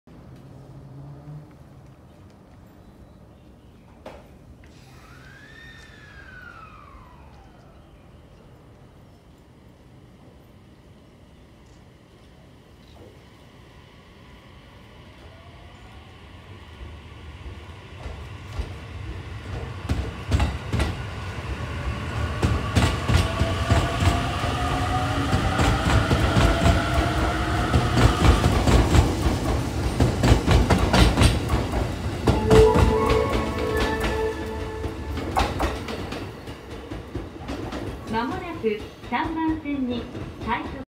【NB-11編成 使用】快速 成田山初詣ぐんま号 成田駅 発車
NB-11編成 使用でした。
成田山初詣ぐんま号は
成田～前橋間 結びます。